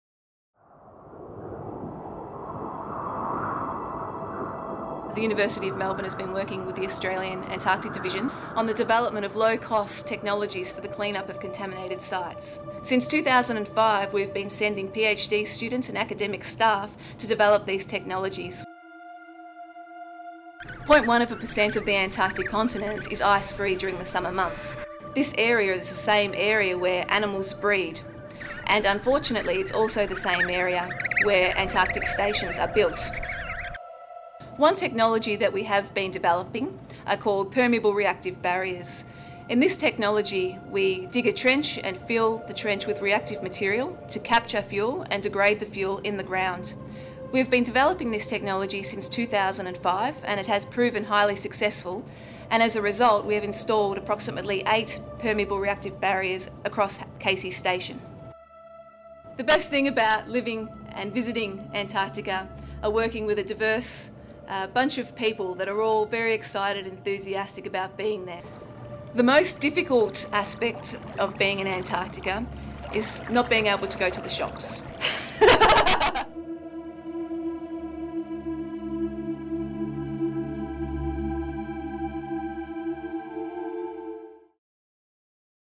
Keeping Antarctica clean
Dr.Kathryn Mumford explains the University of Melbourne's on going mission - cleaning up Antarctica. Video by Paul Burston